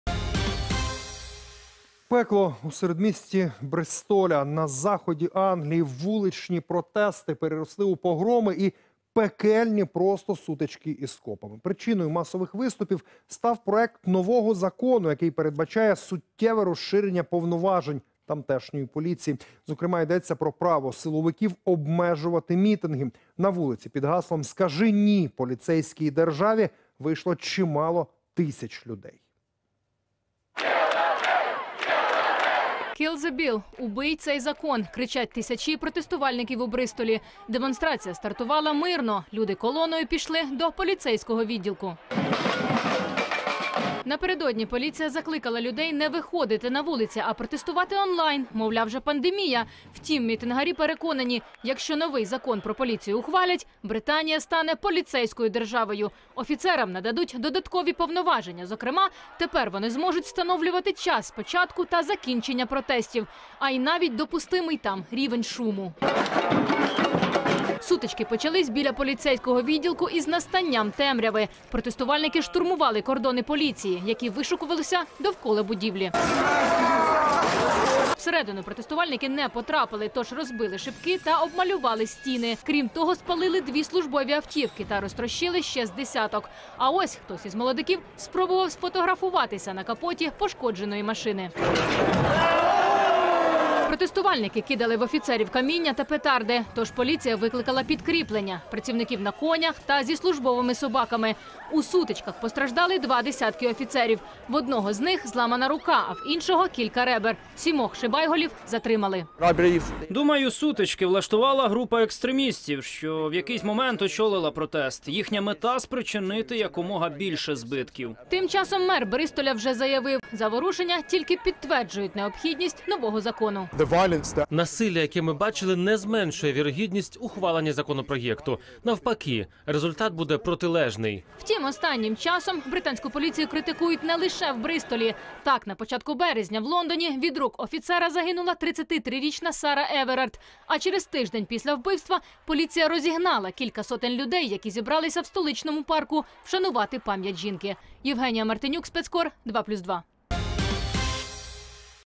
На заході Англії вуличні протести переросли у погроми та сутички із копами
Пекло у середмісті Бристоля. Причиною масових виступів став проект нового закону, який передбачає суттєве розширення повноважень тамтешньої поліції. Зокрема, йдеться про право силовиків обмежувати мітинги.